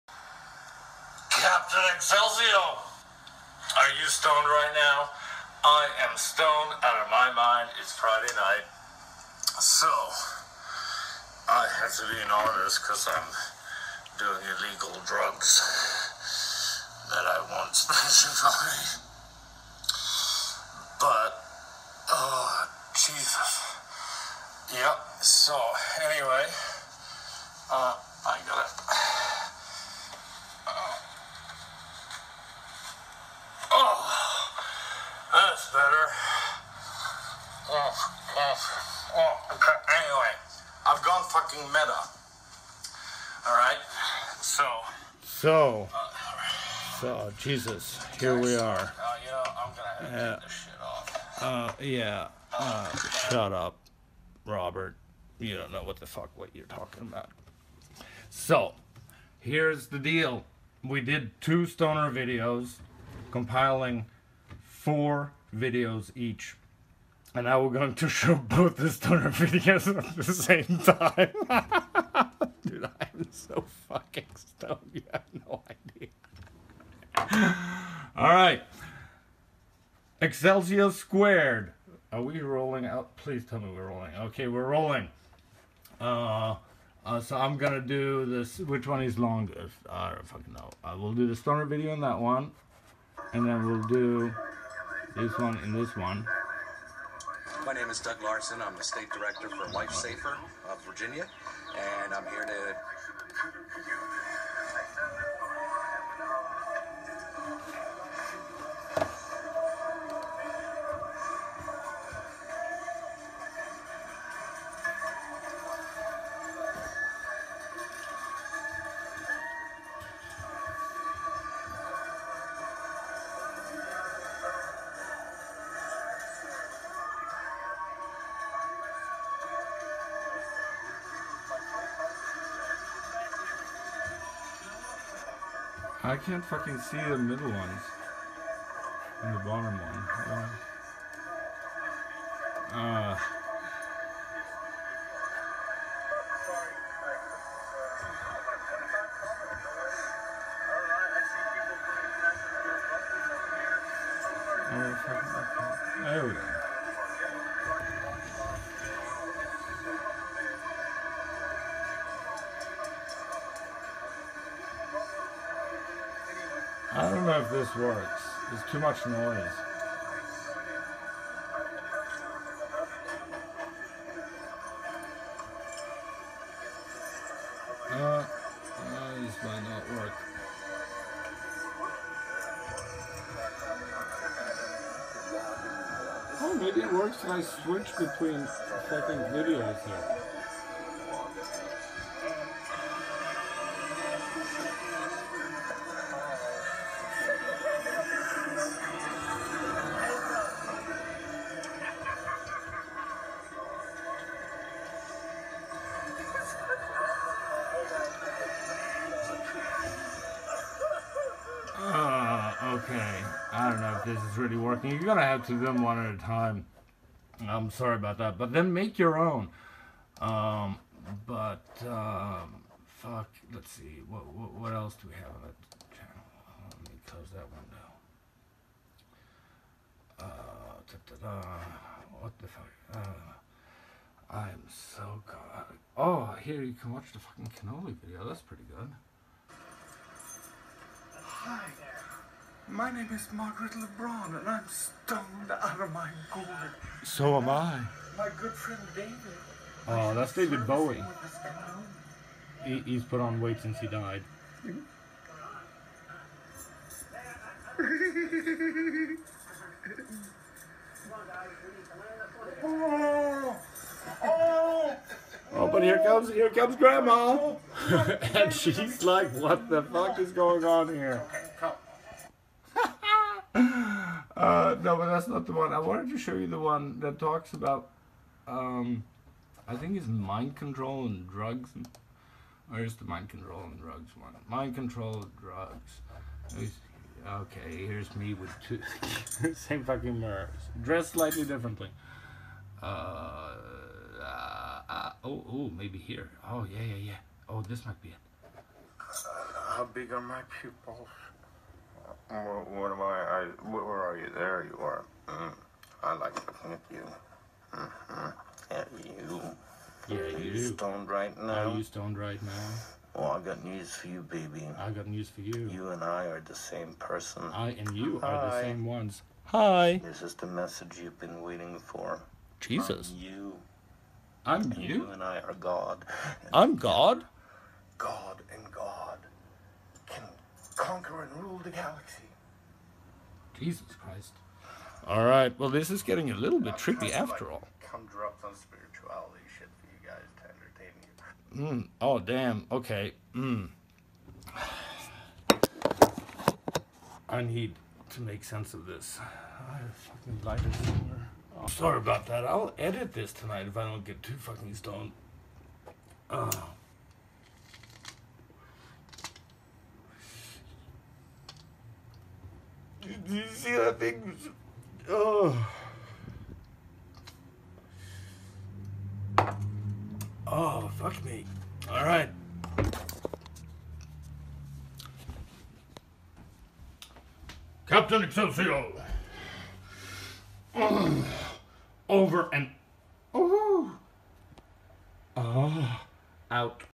Accelcio Meta (We have been hacked!)
Someone who is definitely NOT Accelcio CEO Robert Lagerman takes magic mushrooms, what I *think* were some quaaludes (that I gave someone a blowjob for) and like, dude, 2 grams of medical grade pot and I can barely type this. But this isn't me! I'm Kim Jong-un. I swear it. This is not me typing this right now. Those drugs are, like, friggin' illegal!